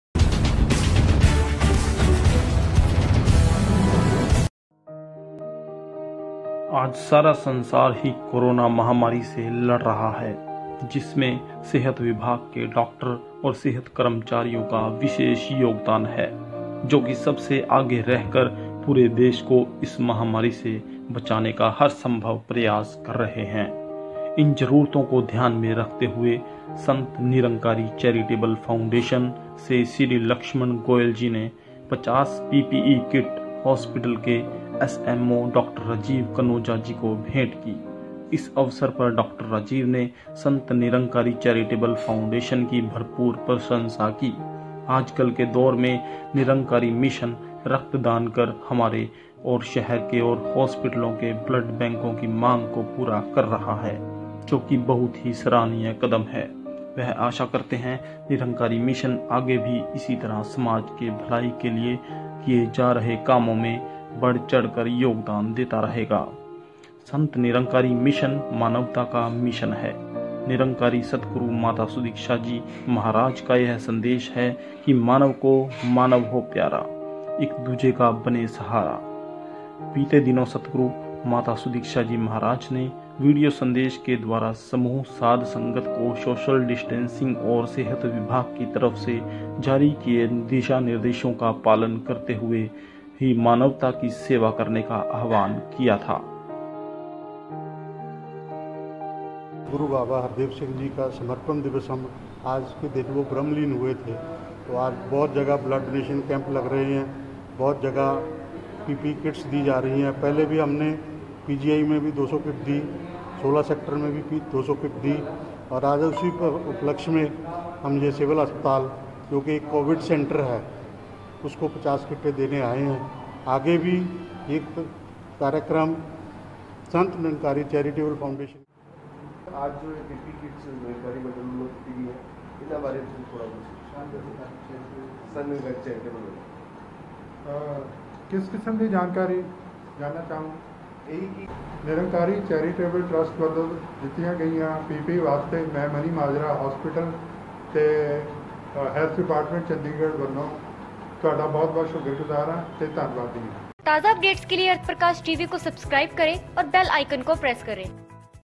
समर्पण दिवस पर सन्त निरंकारी मिशन द्वारा मनीमाजरा अस्पताल भेजी गई 50 PPE kit
समर्पण दिवस पर सन्त निरंकारी मिशन द्वारा मनीमाजरा अस्पताल में भेजी गई 50 PPE kit